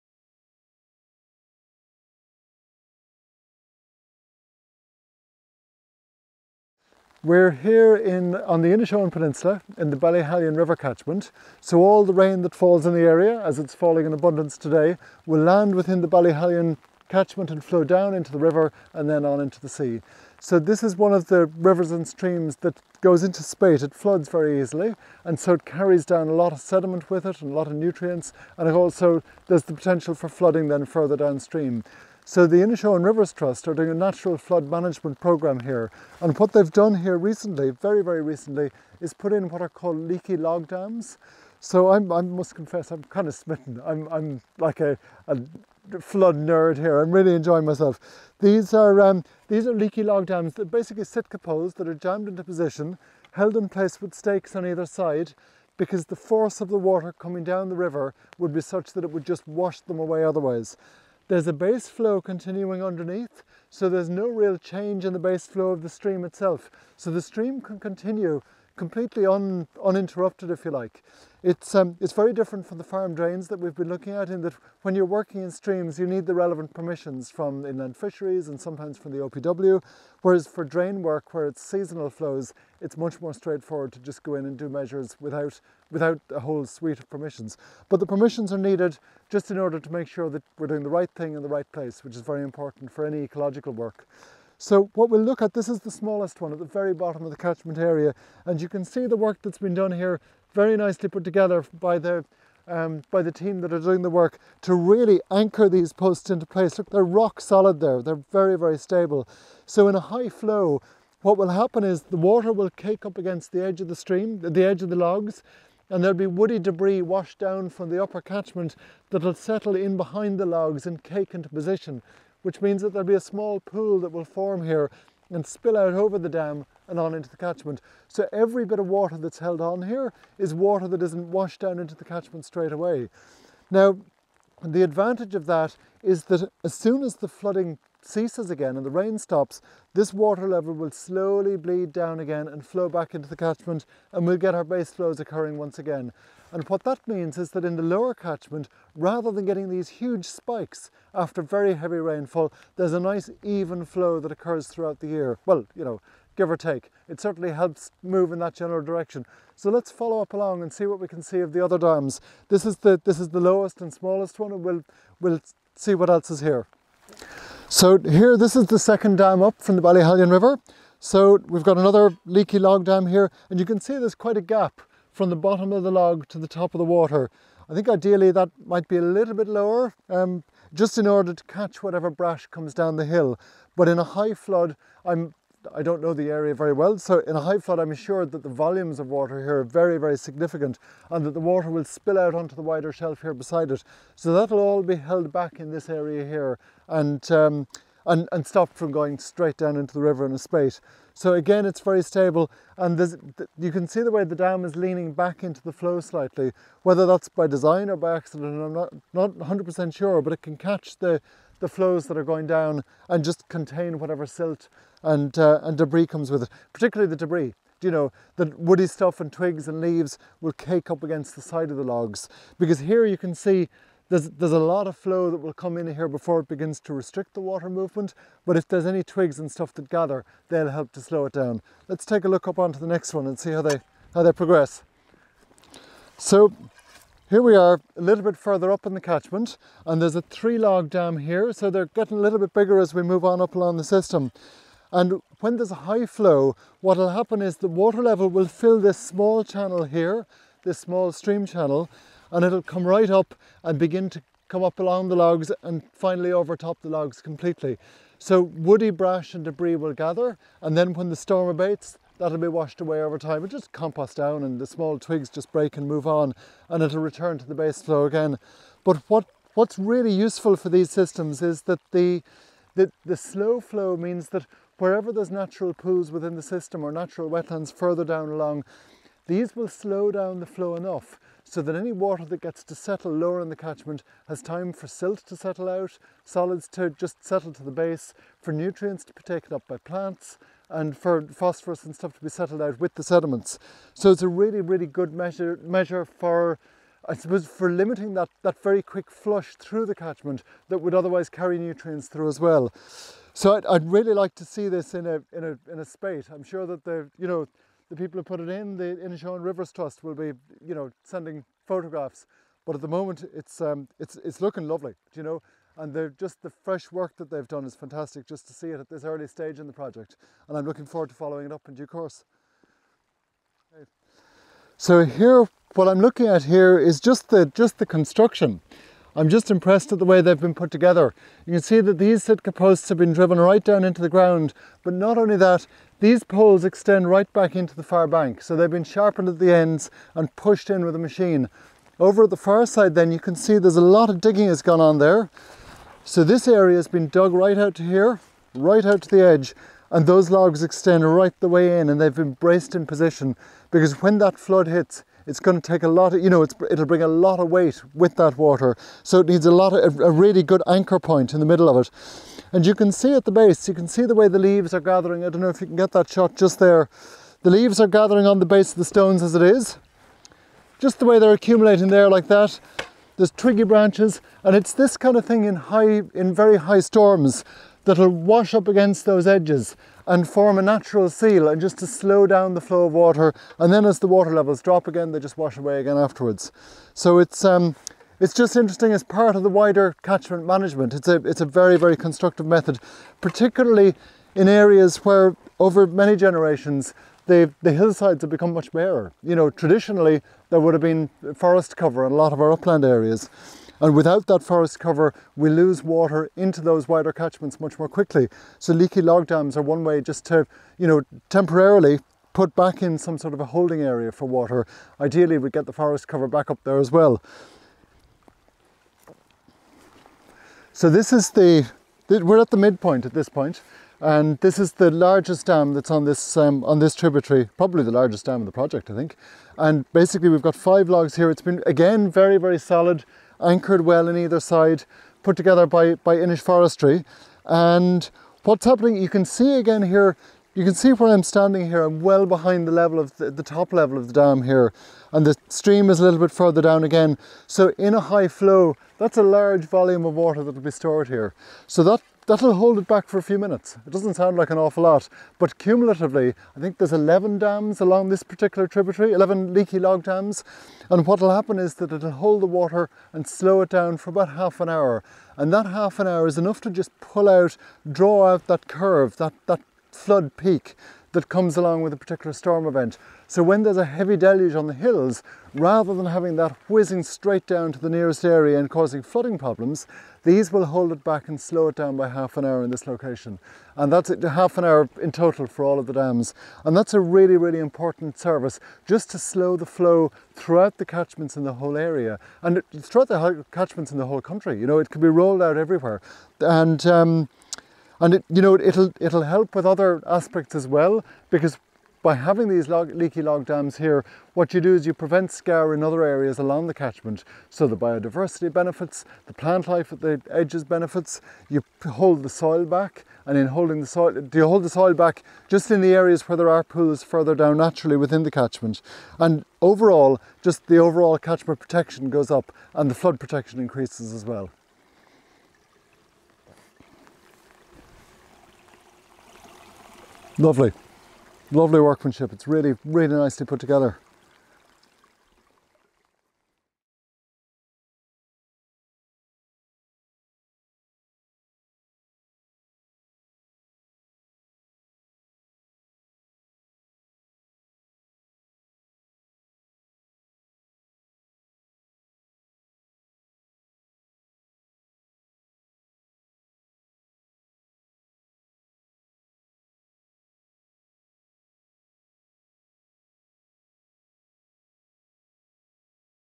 Leaky Dams Clonmany, Inishowen
Wetlands expert Féidhlim Harty (FH Wetlands Ltd) visits Inishowen, Co. Donegal  to advise on nature based solutions for farms, ponds and forests. In this video Féidhlim visits the Inishowen Rivers Trust natural flood management project in Clonmany to see the newly installed leaky dams. These dams aim to slow the flow of water through the Ballyhallan River catchment and form part of a network of dams on streams and drains. Féidhlim explains how the dams work.